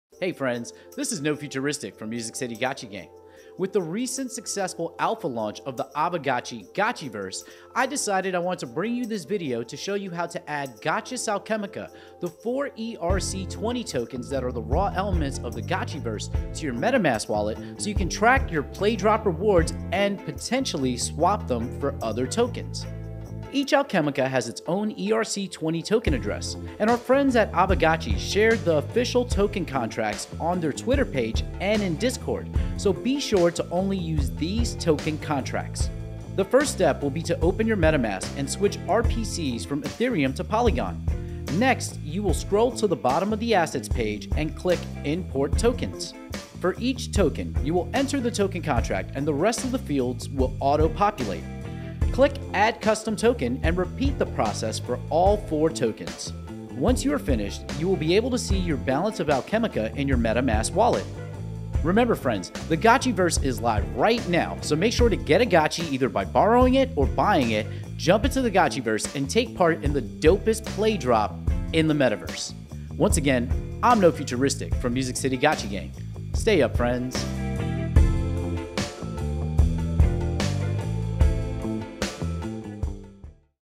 How to Add Gotchus Alchemica To MetaMask
With the successful Alpha Launch of the Aavegotchi Gotchiverse, I made this video showing the gotchigang how to add Gotchus Alchemica to their MetaMask wallet so they can see the their balance of rewards